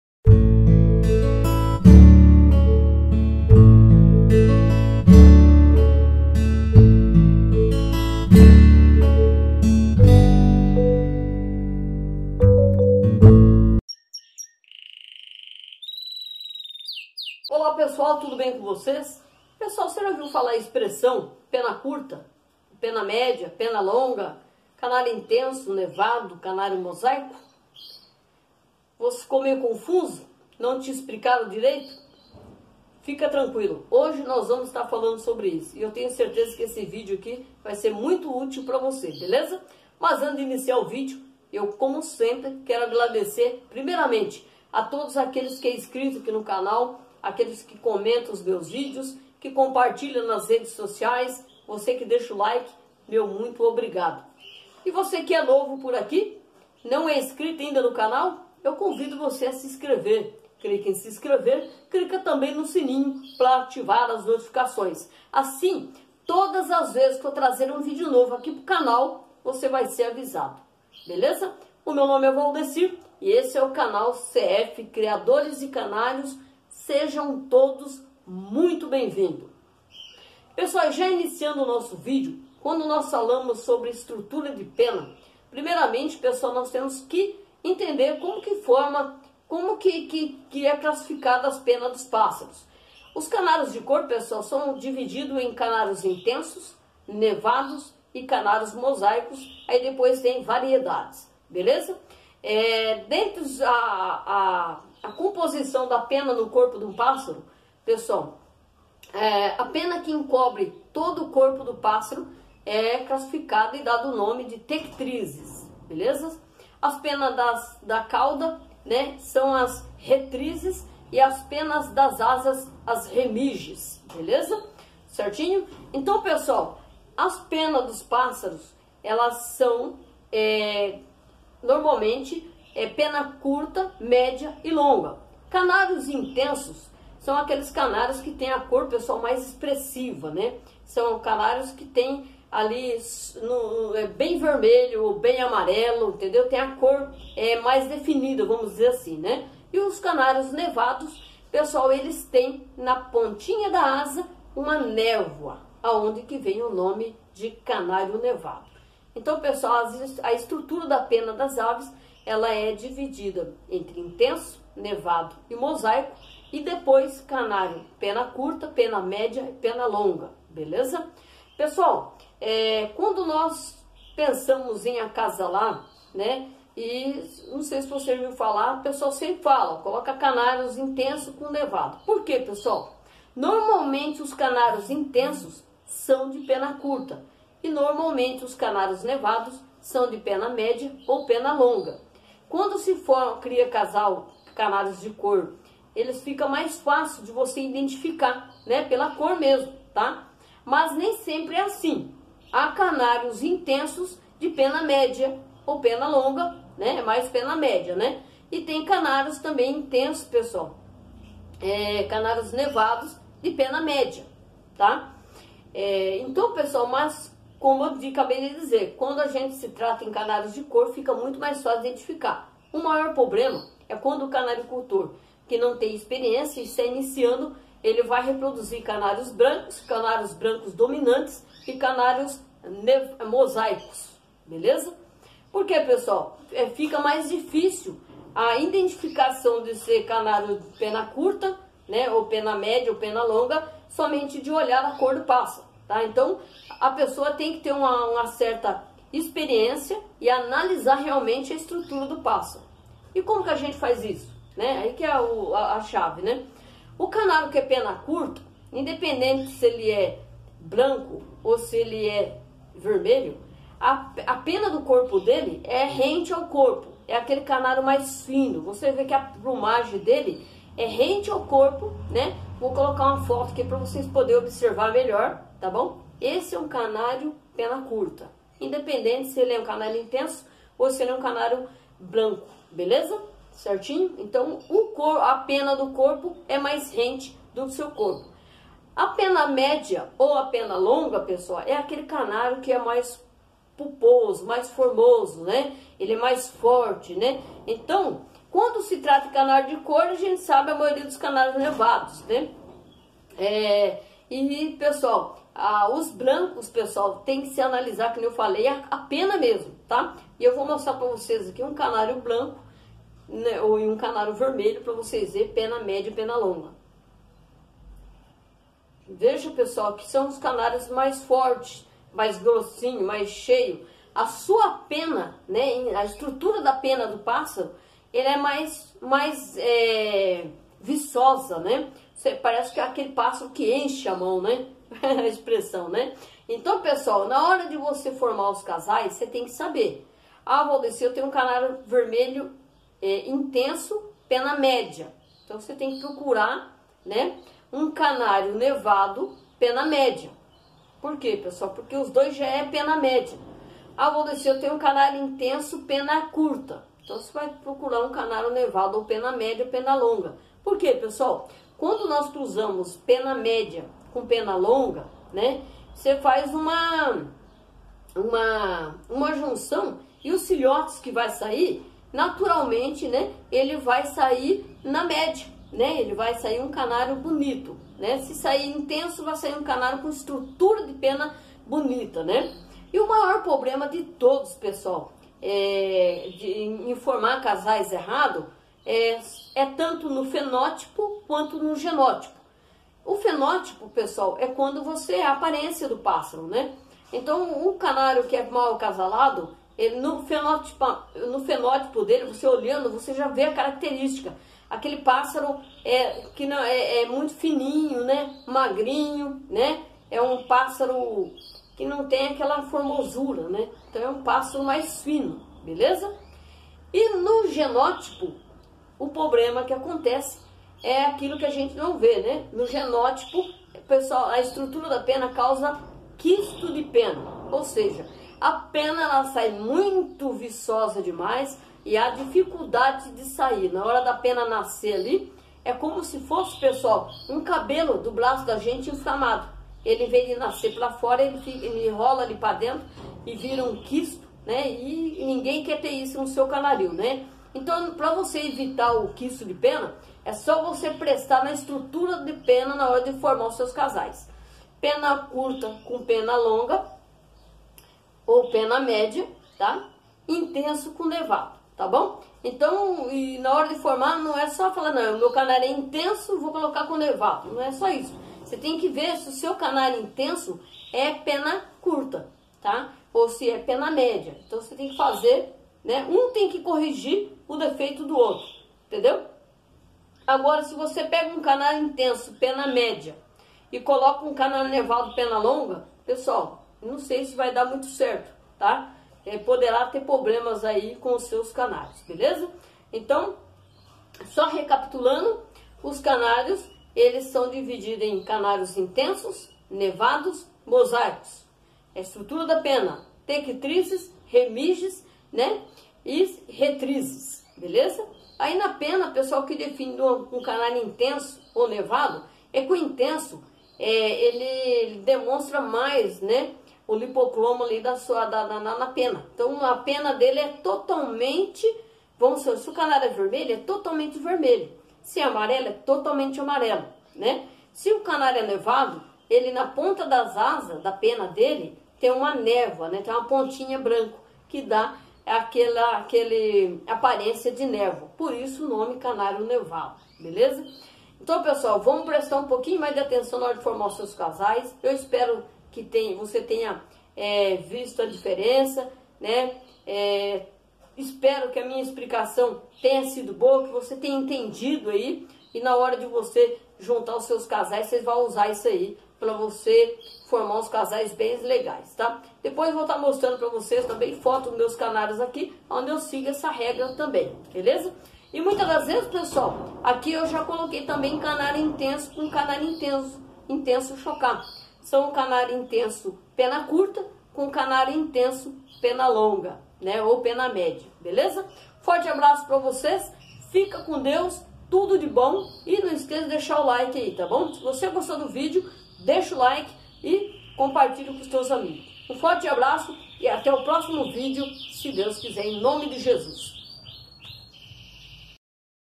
VOCÊ TEM QUE SABER DISSO NA HORA DE FORMAS OS CASAIS
Nesse video  eu tendo passar de forma simples como devemos analizar as extruturas das penas antes de formar os casais, espero que esse video seja útil,  obrigado a todos